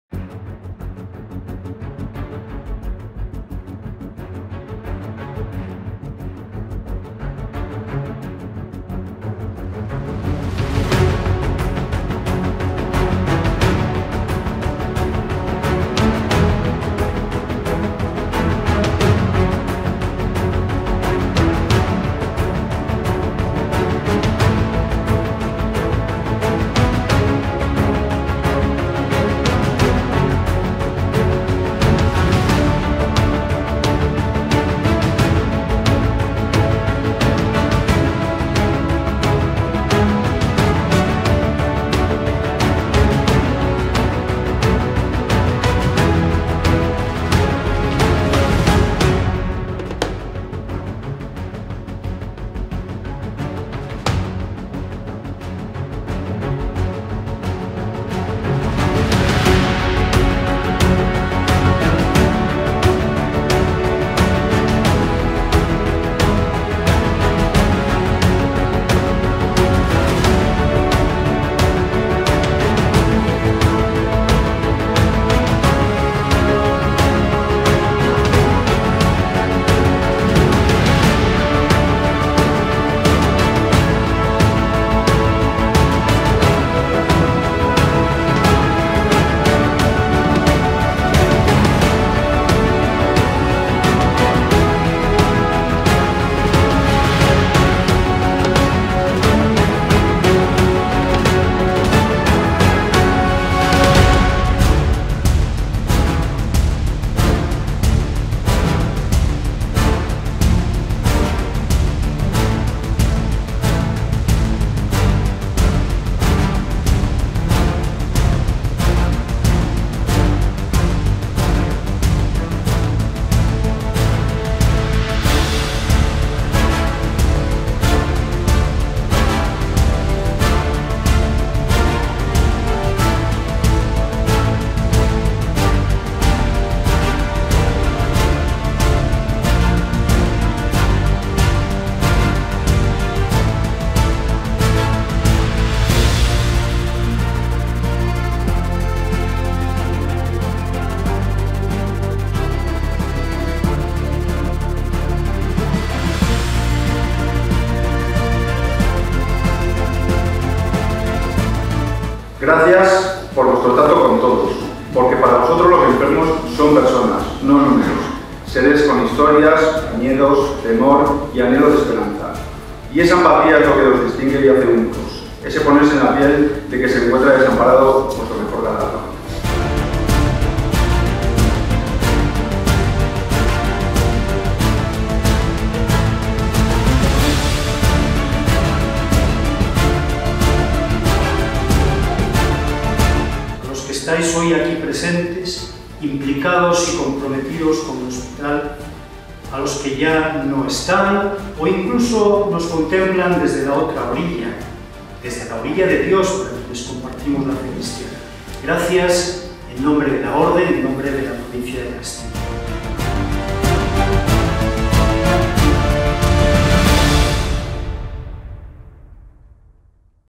50 Aniversario del Hospital San Juan de Dios de León
El Hospital San Juan de Dios de León, perteneciente a los Hermanos de San Juan de Dios, ha trabajado desde su apertura para convertirse, 1968, en una referencia de la atención sanitaria privada de calidad en la provincia, contando con los mejores profesionales, invirtiendo de manera continua en tecnología, innovación e infraestructuras, apostando por la calidad y con la hospitalidad como valor fundamental. Esa sólida reputación que ha ido construyendo el Hospital San Juan de Dios de León se ve reflejada en una medicina excelente, segura, humana e innovadora que da la confianza que necesitan sus pacientes, tal y como indican los índices reflejados en las encuestas de satisfacción que se realizan a lo largo del año.

Si tienes interés en conocer el Hospital San Juan de Dios de León puedes acceder a su página web

La música de este video ha sido descargada de la web